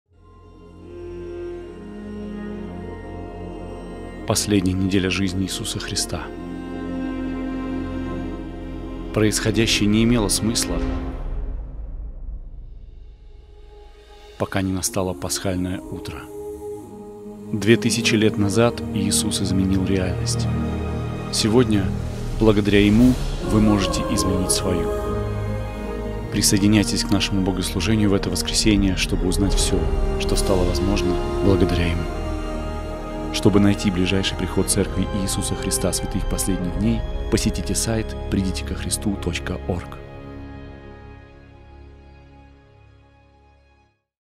Вы можете, благодаря Ему
В последнюю неделю земного служения Иисуса Христа мир был полон хаоса. Но благодаря Его жертве, совершенной 2000 лет назад, сегодня мы все можем чувствовать бесконечный покой. На этой неделе вы сможете ощутить Его мир, прославляя Спасителя вместе с нами в церкви. Присоединяйтесь к нам в Пасхальное воскресенье, когда мы собираемся, чтобы петь, молиться и поклоняться Иисусу Христу в часовнях по всему миру. Посетите чтобы найти ближайшую к вам общину!